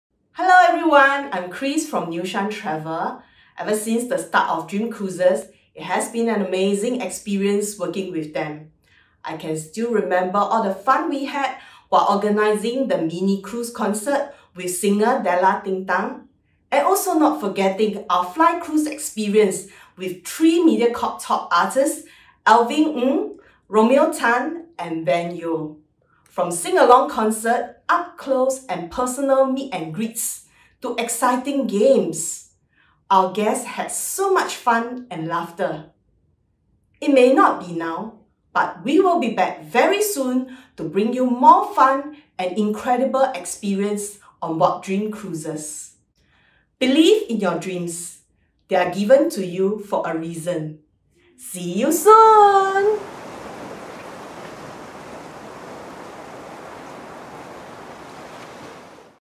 Dream Cruises - SeaYouSoon New Shan Travel Chris Tay
【SEAYouSoon Missing all the precious memories at sea 】

Watch as Chris Tay New Shan Travel shares her most memorable moments on DreamCruises. What are the precious memories you had with your loved ones at sea? Share with us in the comment section! SeaYouSoon!